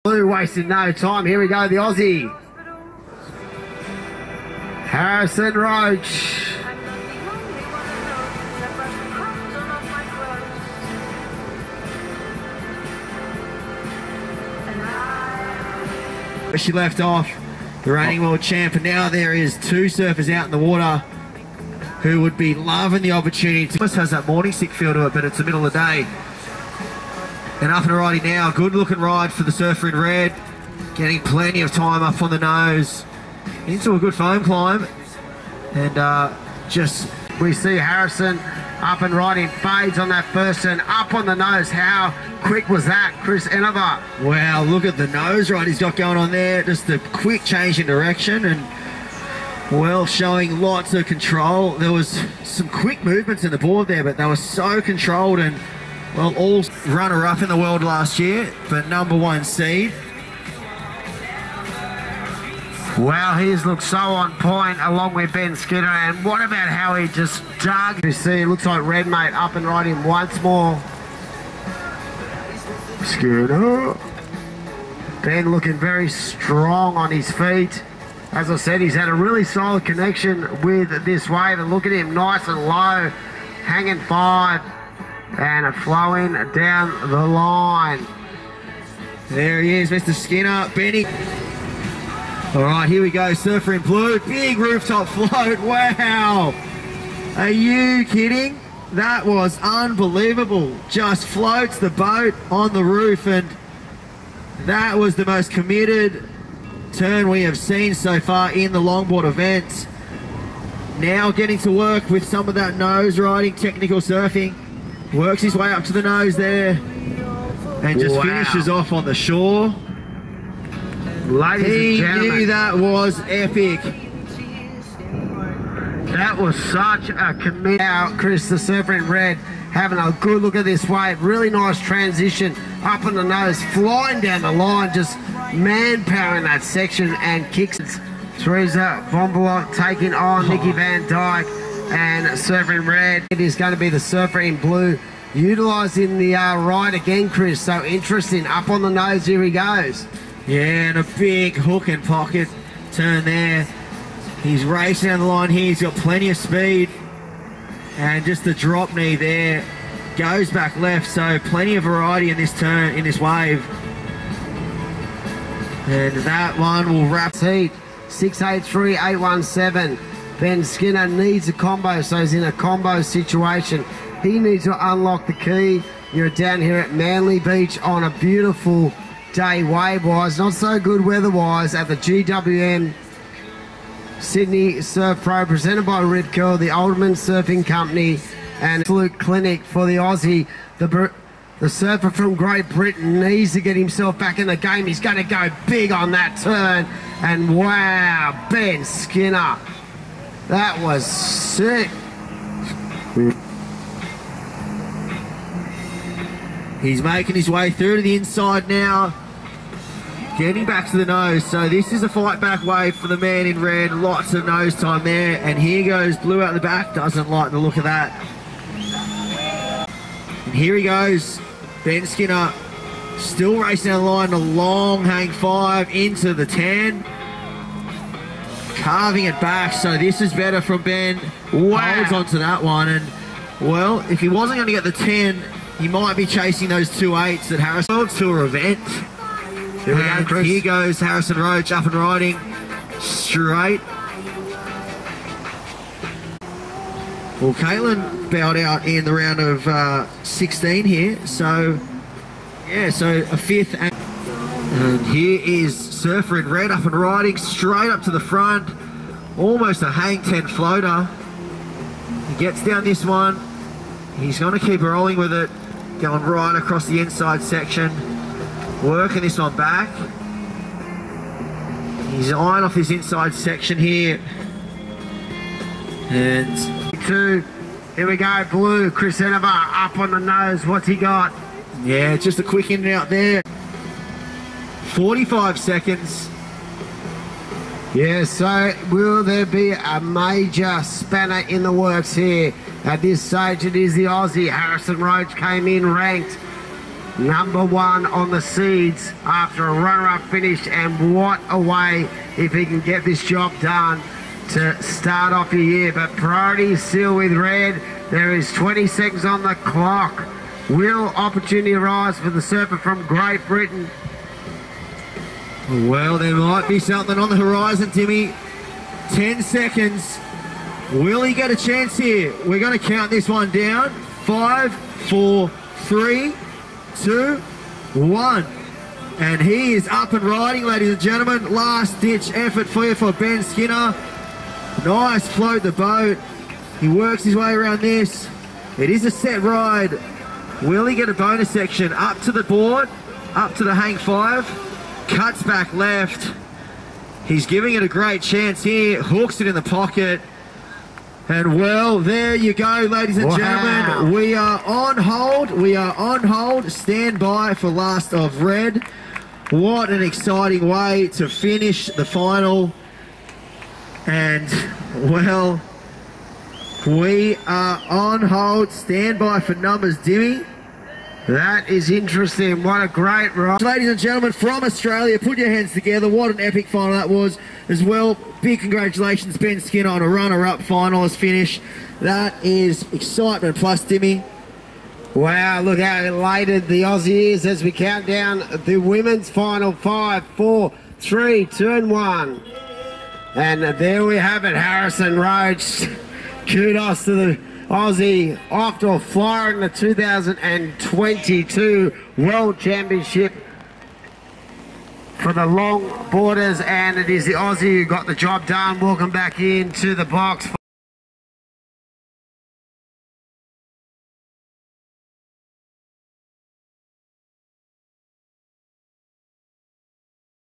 WSL Longboard - Mens final. Ben Skinner vs Harrison Roach.
24 May 2022
Manly Beach, Australia.
Highlights of the Mens Final, Ben Skinner (UK) vs Harrison Roach (Australia)
Very raw , no music, just the on-beach commentary.